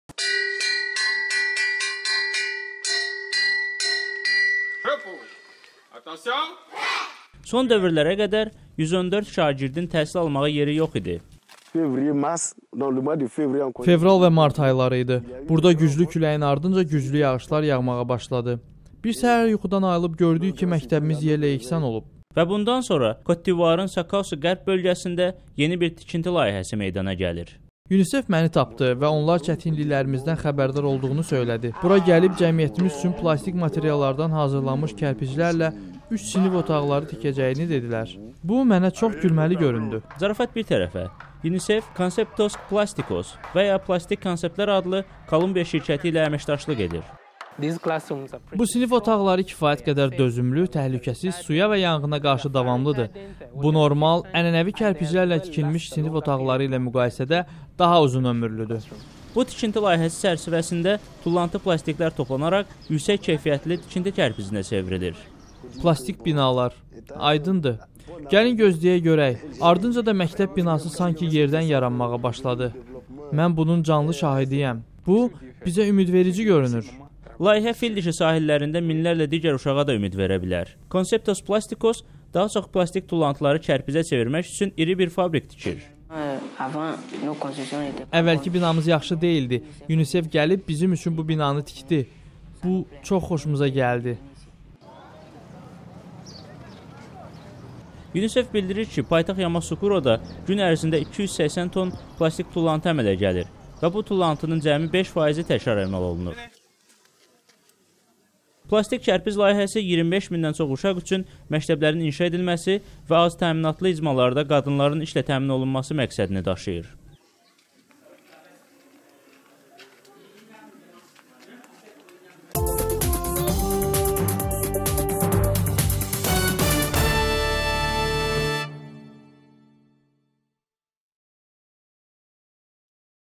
Plastik tullantılardan məktəb inşası
Ekoloqlar su yollarını tutan və bütün dünyada ekosistemləri təhdid edən plastik tullantıları təmizləmək üçün yaradıcı həll yolları tapmağa çağırır. Birləşmiş Millətlər Təşkilatının Uşaq Fondu Kolumbiyanın bir şirkəti ilə birlikdə Afrikada plastikdən hazırlanmış materiallardan məktəb inşa etməyə başlayıb.